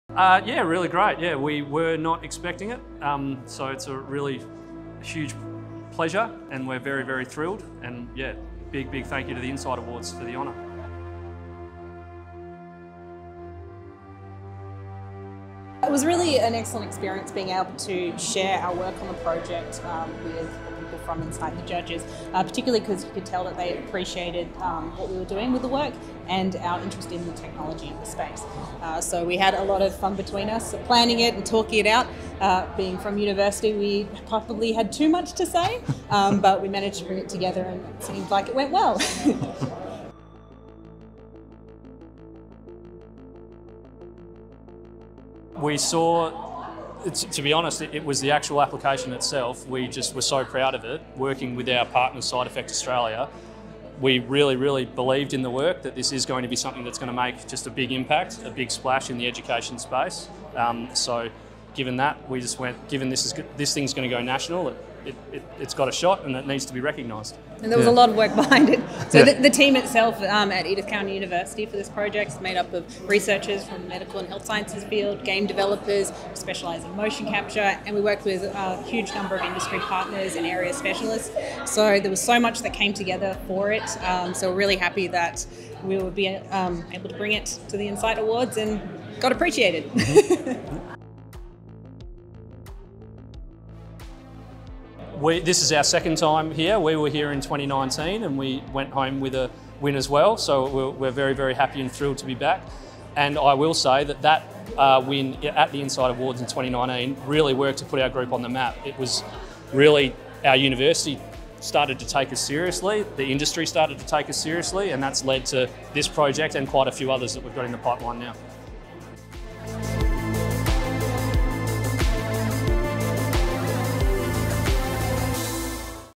Two minutes with 31st INCITE Awards winners, Dr Brennen Mills & Jessica Watson
We chat to Dr Brennen Mills & Jessica Watson from Edith Cowan University, 2022's Research & Innovation Project of the Year winners, about their INCITE experience.